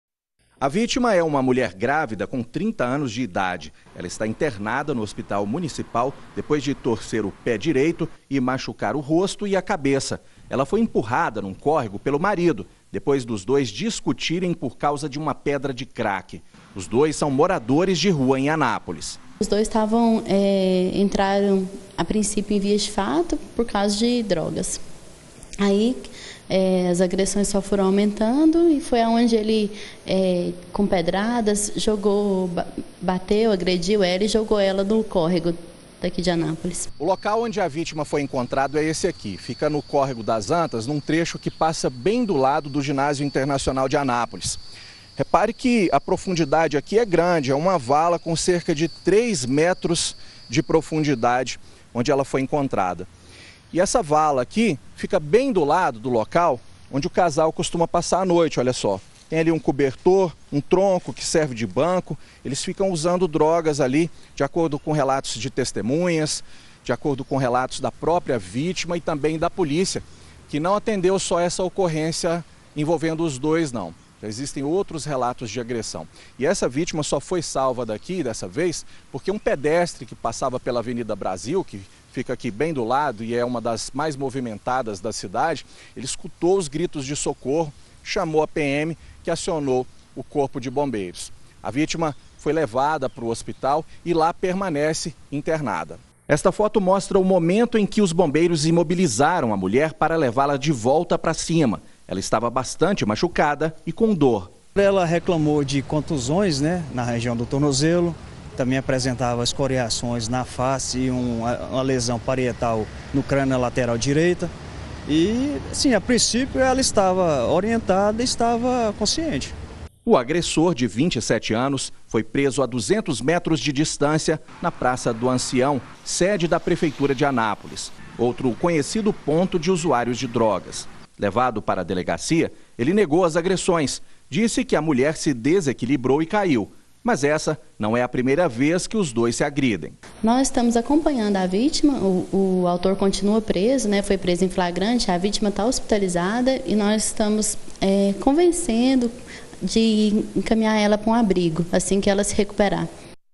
BG - Homem empurra grávida em córrego - 01-10-2018
Um morador de rua foi preso depois de empurrar a mulher grávida pra dentro de um córrego em Anápolis.